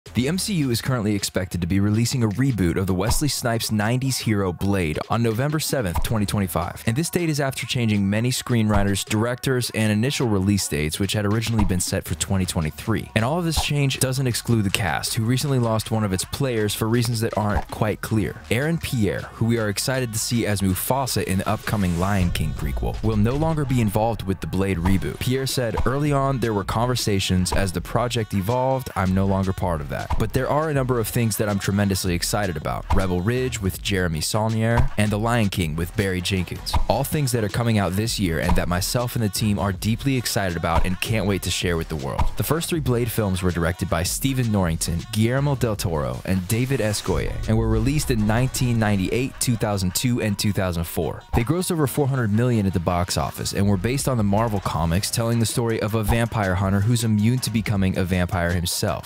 Blade Actor Drops Bombshell: Out of Marvel Reboot!
Description:

Dive into the latest news as a Blade actor reveals unexpected details about their involvement in the upcoming Marvel reboot.